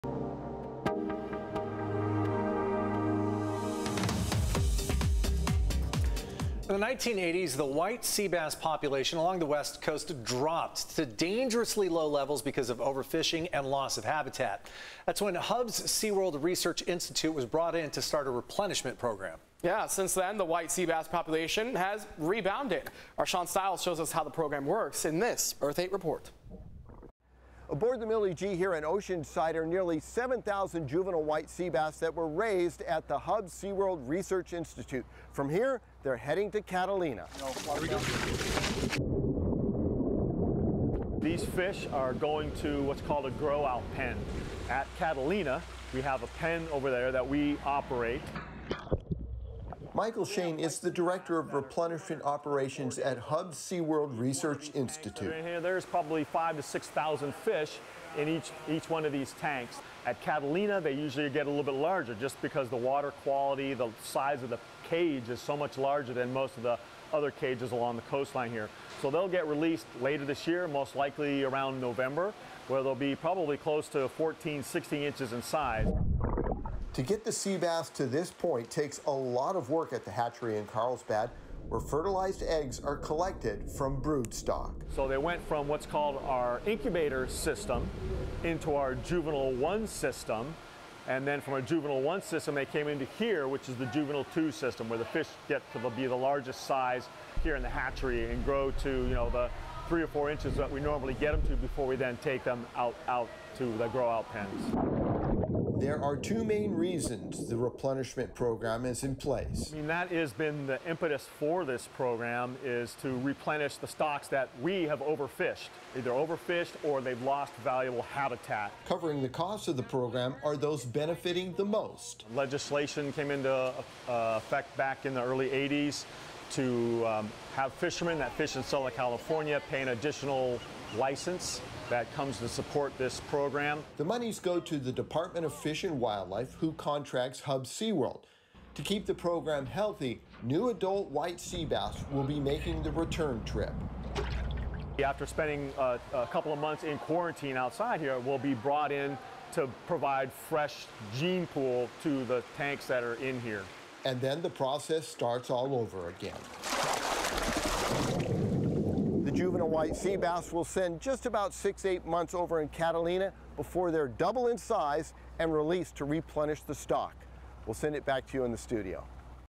San Diego research institute growing White Seabass population off California coast
The fish begin in Carlsbad before making a trek to Catalina to grow. Once they reach the right size the fish are brought back as a part of the replenishment program.